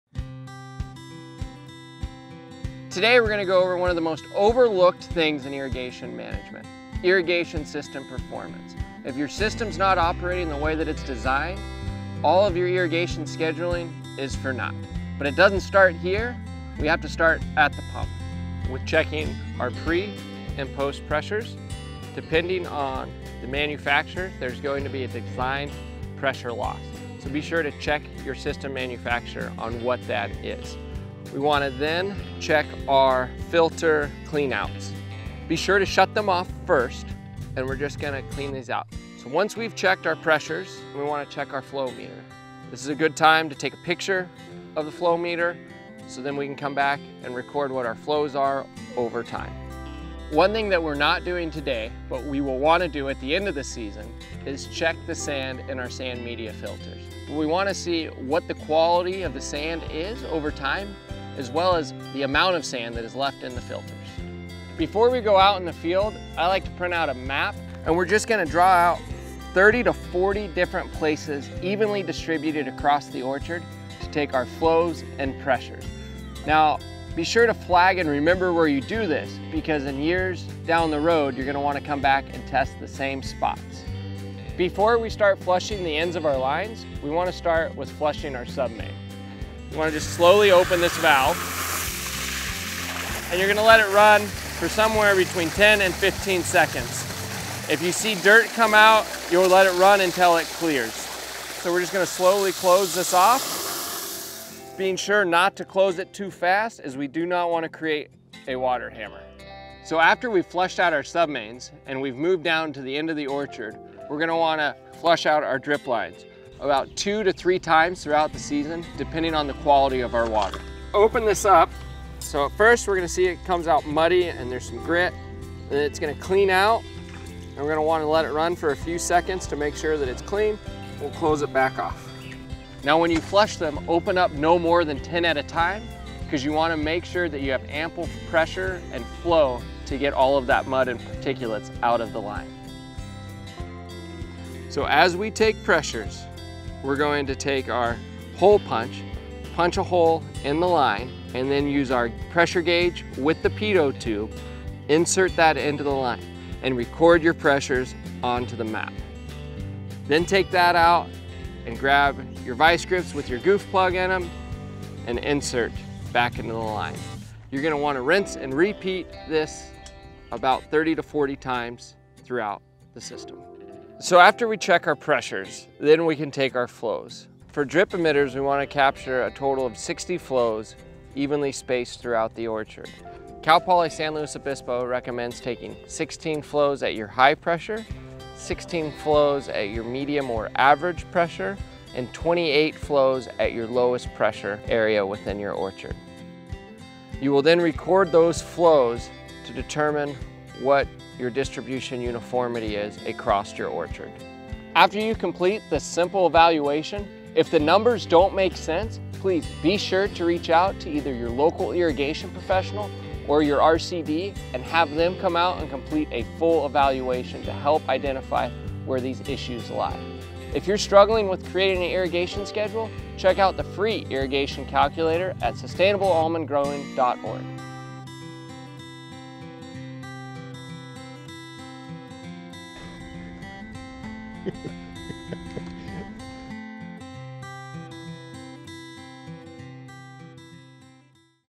The Irrigation Station | Irrigation System Performance | California Almonds
In the latest installment of The Irrigation Station, ABC provides insight into one of the most overlooked aspects of irrigation management: Irrigation System Performance. After watching this episode, use the free irrigation calculator at SustainableAlmondGrowing.org to create your own irrigation schedule and consult pages 66-87 of the Almond Irrigation Improvement Continuum at Almonds.com/Irrigation for more information about calculating irrigation system performance.